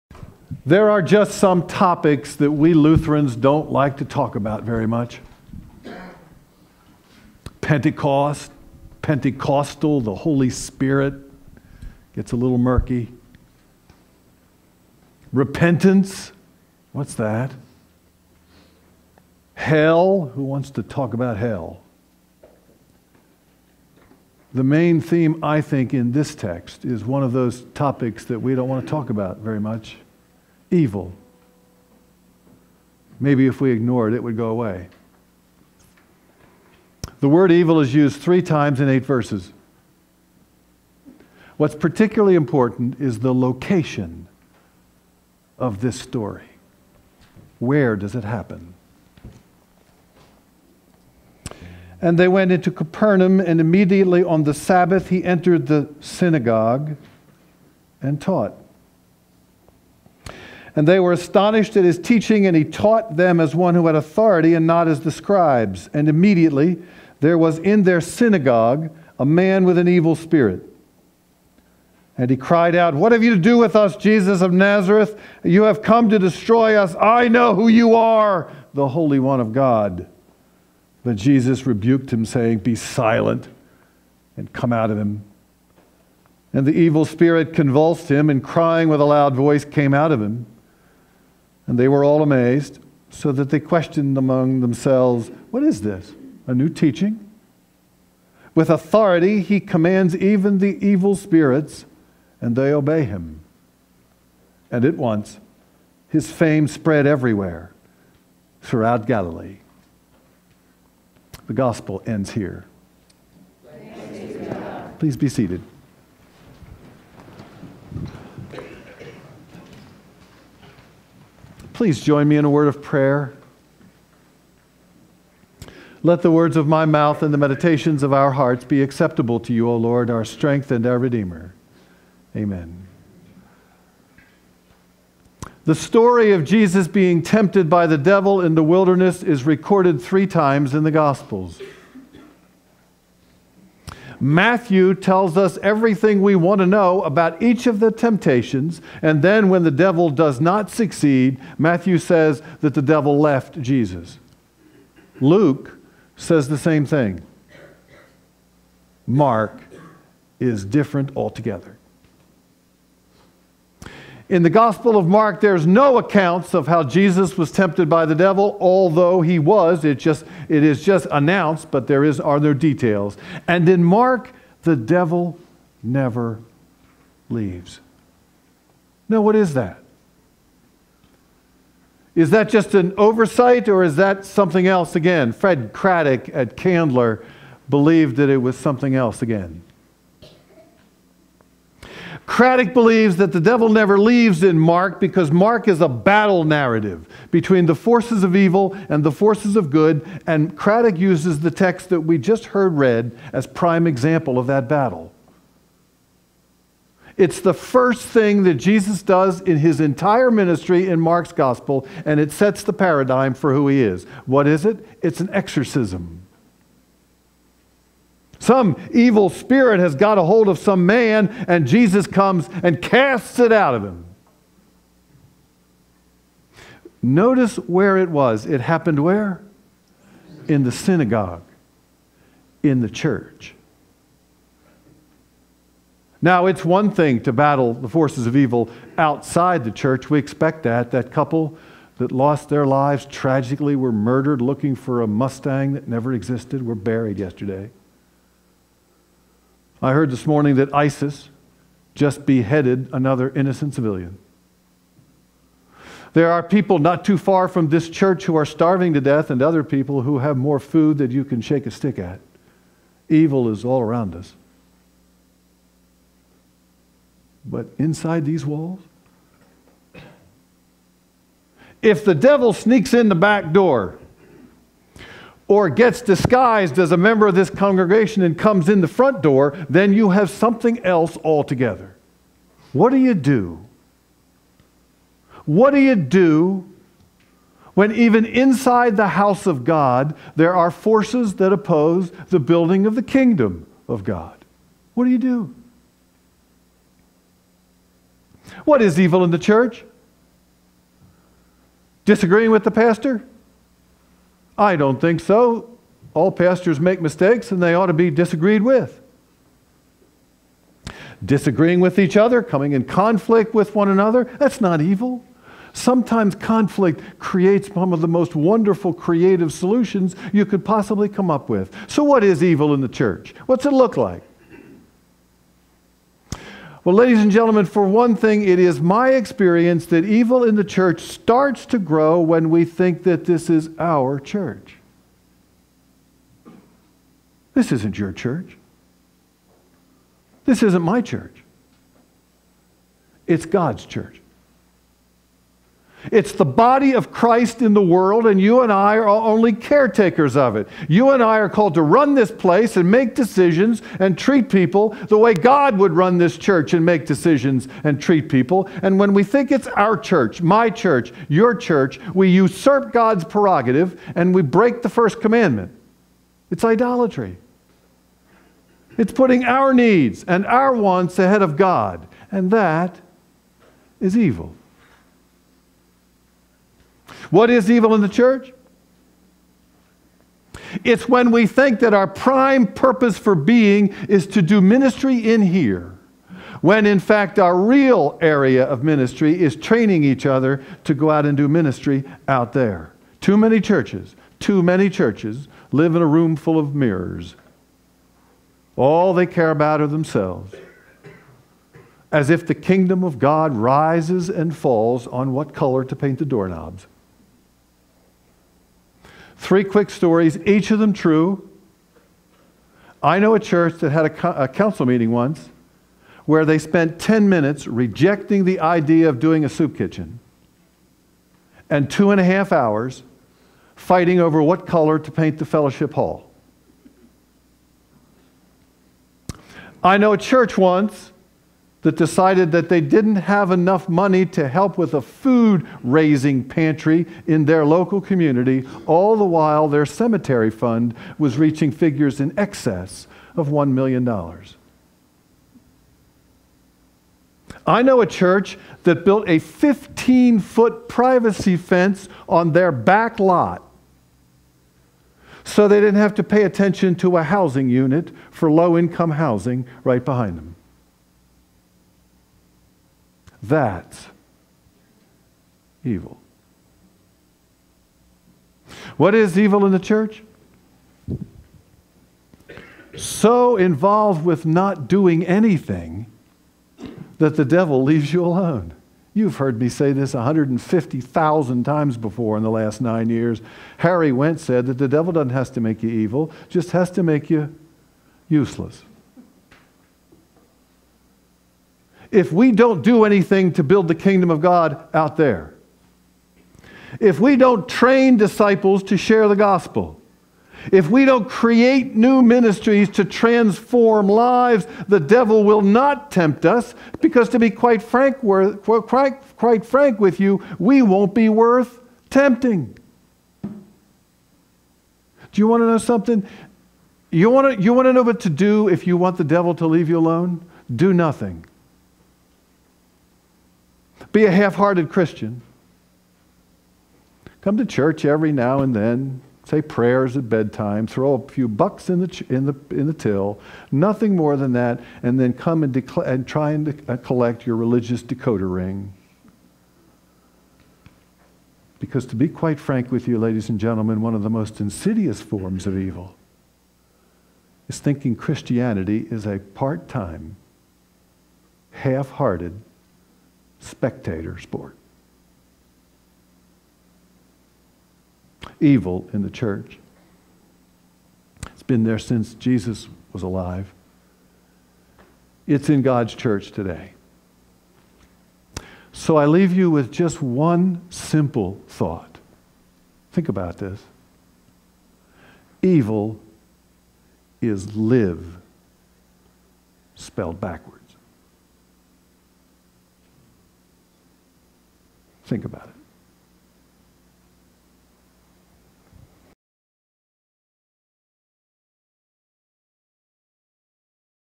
LCR Sermon 2-1- 2015
Sermon (("Repentance at LCR - Part 2") by Senior Pastor Dave Helfrich at the Lutheran Church of the Resurrection, Marietta, Georgia on February 1, 2015.